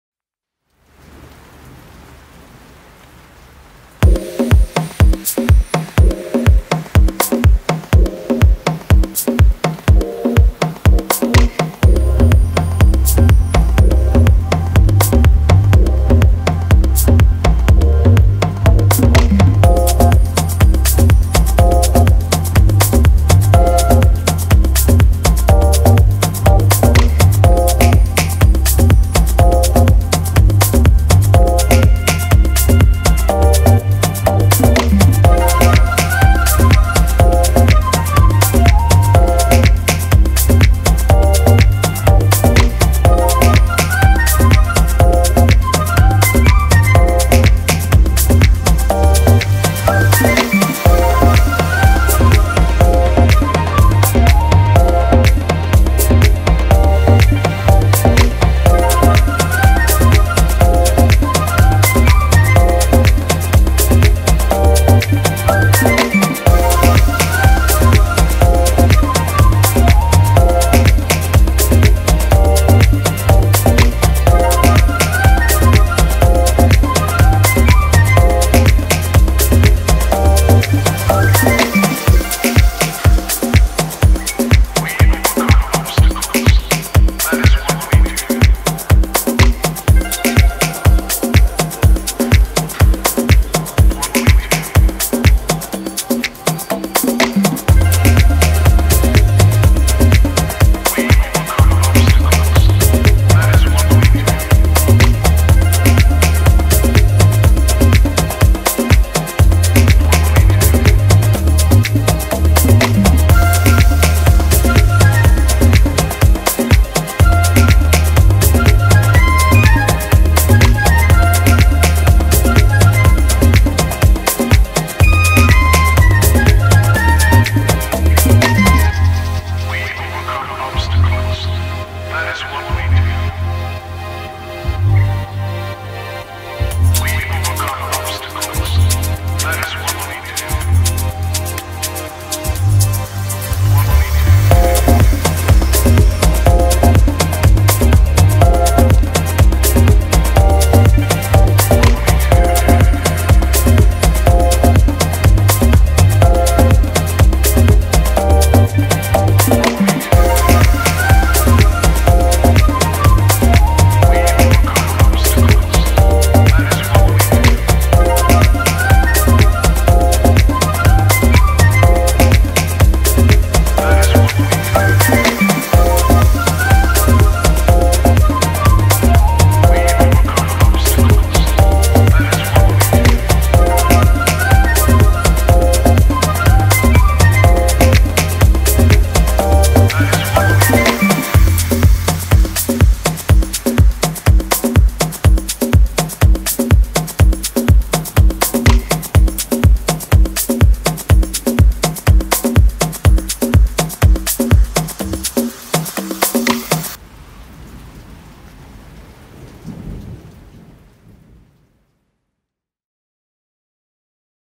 Tulum, Mexico
Tulum, Mexico.  Traveling to a boho chic paradise during a pandemic.